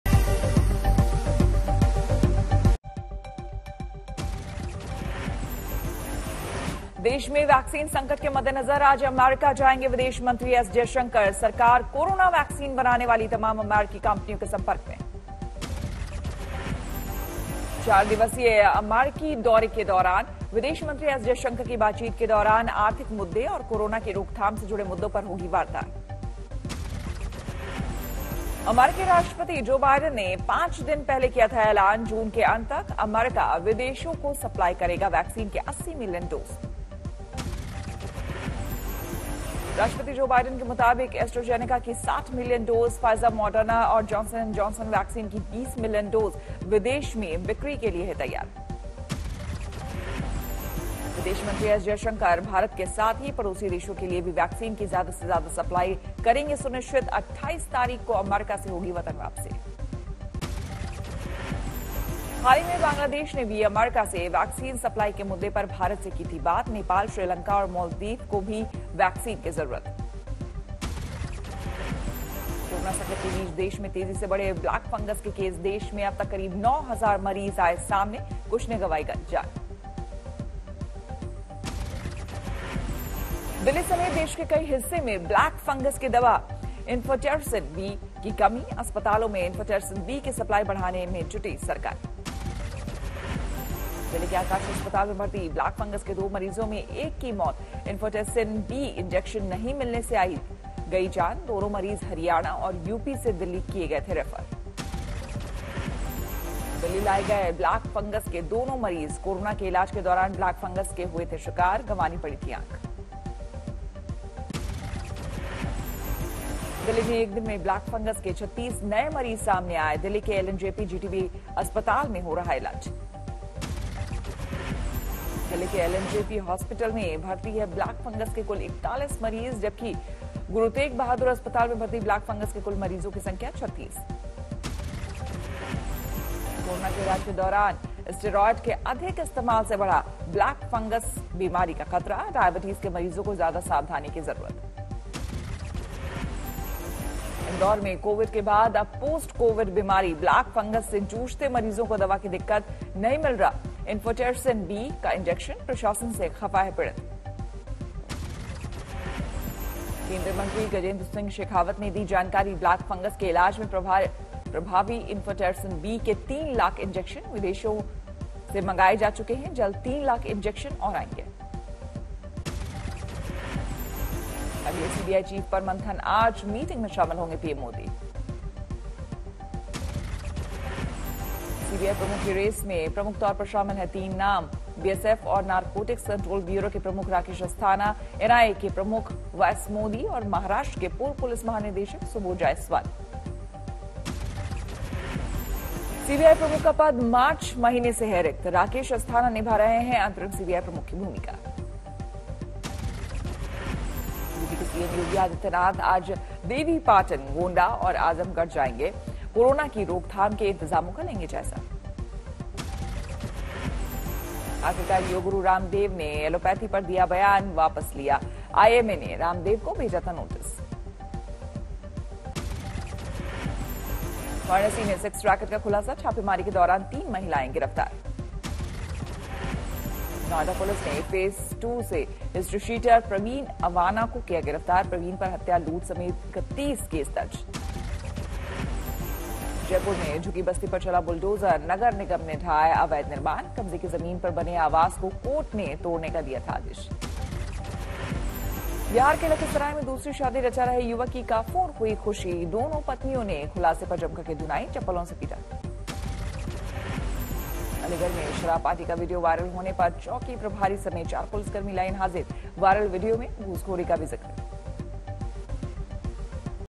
Corona Vaccine के आज America जाएंगे विदेश मंत्री S Jaishankar | देखें सुबह की 25 बड़ी खबरें
Coronavirus Vaccination: देश में कोरोना वैक्सीन की कमी को देखते हुए केंद्र सरकार ने विदेश मंत्री एस जयशंकर को अमेरिका भेजने का निर्णय लिया है. एस जयशंकर अमेरिका की वैक्सीन निर्माता कंपनियों और शीर्ष अधिकारियों से भारत में वैक्सीन की सप्लाई को लेकर चर्चा करेंगे. एस जयशंकर 24 मई से लेकर 28 मई तक अमेरिका के दौरे पर रहेंगे. इस दौरान वो भारत और पड़ोसी देशों में ज्यादा से ज्यादा वैक्सीन की सप्लाई के लिए अमेरिका के साथ बातचीत करेंगे...